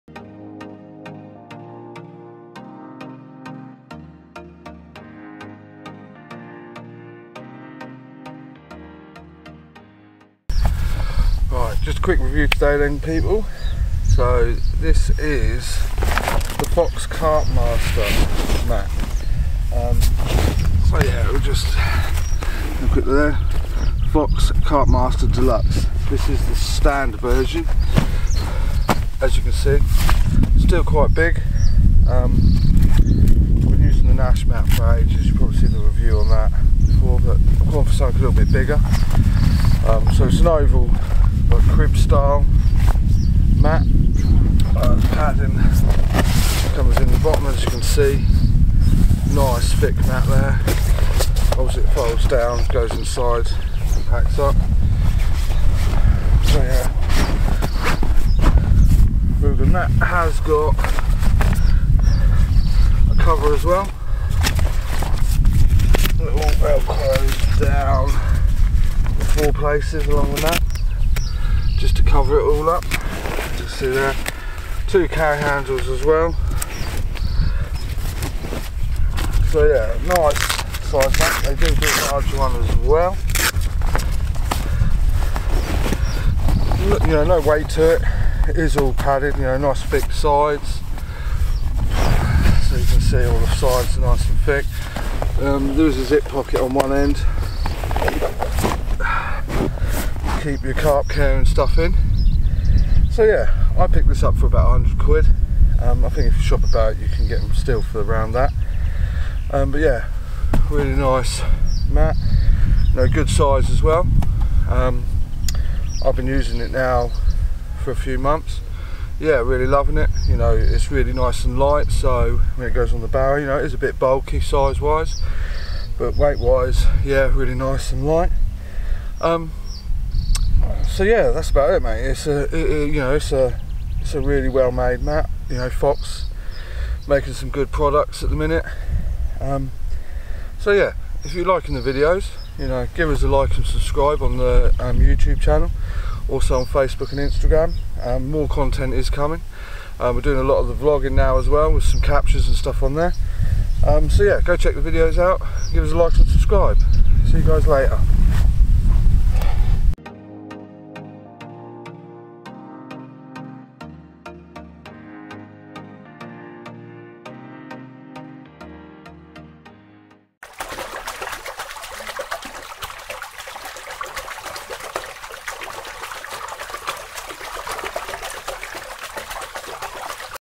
CARP FISHING REVIEW FOX CARP MASTER DELUXE MAT
QUICK REVIEW OF THE FOW CARPMASTER DELUXE MAT
Ive used for a few months and love it